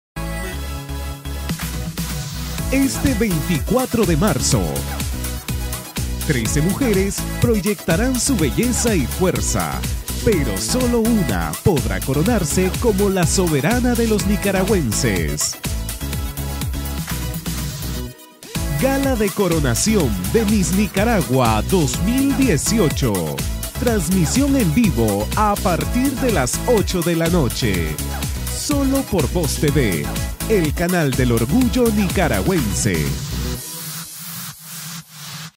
Gala de Coronación Miss Nicaragua 2018
Este 24 de marzo, 13 mujeres proyectarán su belleza y su fuerza, pero solo una podrá coronarse como la nueva Miss Nicaragua 2018. Vos TV, el canal del orgullo nicaragüense, transmitirá la gala en vivo, a partir de las 8:00 de la noche.